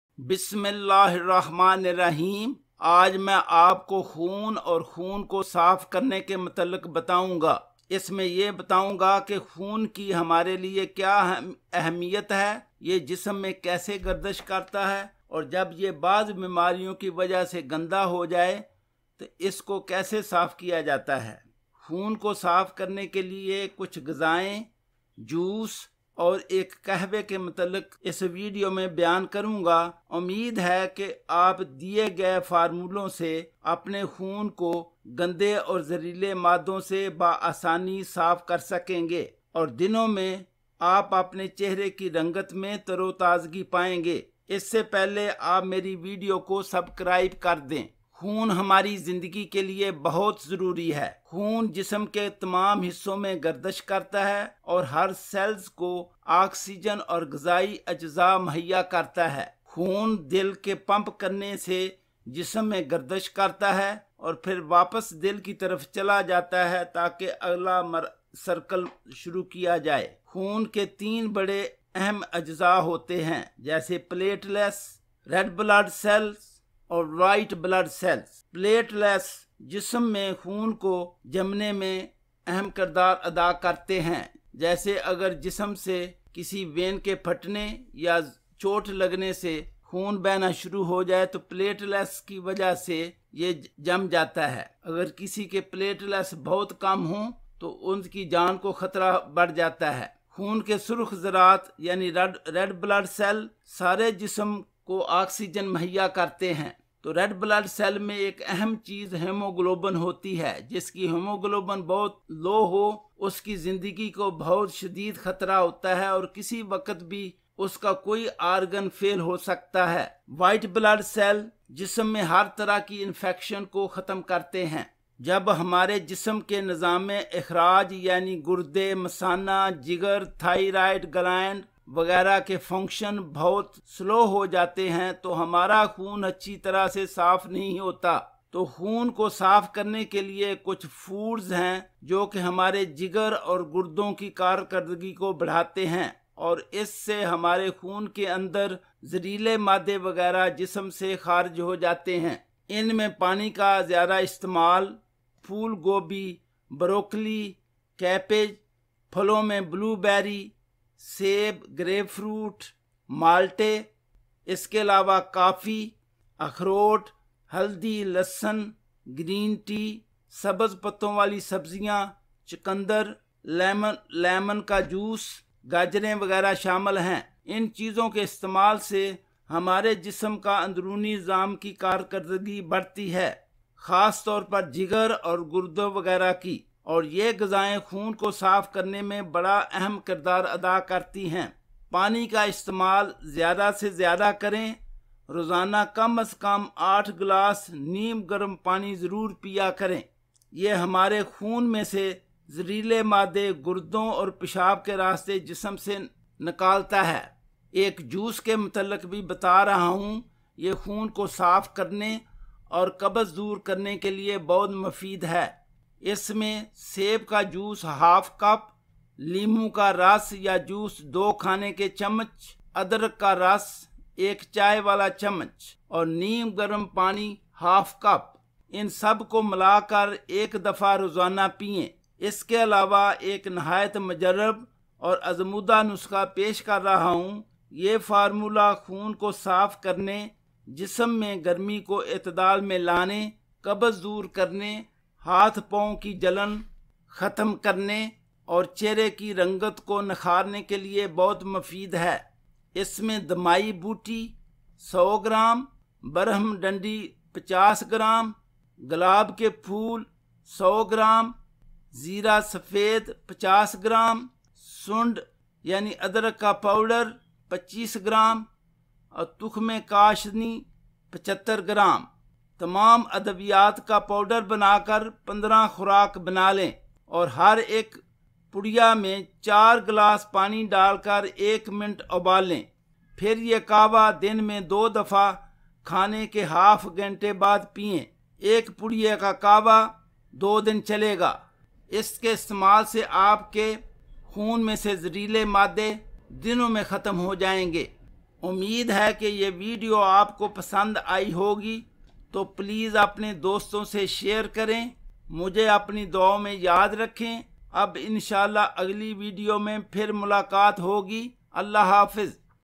Purify Your Blood For Glowing And Ageless Skin | Why blood gets impure | Dr. Shad
Assaalamu Alayacum, I am Doctor Fakhar Shad and my channel name is SHAD CLINIC. In this video I explained, how we can purify our blood naturally and make our skin glower and younger. I described the importance and structure of blood. Blood is essential to our life. It circulates through the body and delivers essential substances like oxygen and nutrients to the body's cells. I explained why our blood gets toxic and importance of water that most common and simplest natural blood purifiers. Kidneys flush out toxins from the blood through urine and water is the best to helpful in this process. I introduced a homemade fresh juice with apple, lemon and ginger. If this type of juice drink only one weak, your face color would glow and look younger. An another drink (kawa) with several herbs which detox your blood with in 30 days. This drink(kawa) also lower your heat palpitation from body, hands, and feet as well as  eliminate constipation.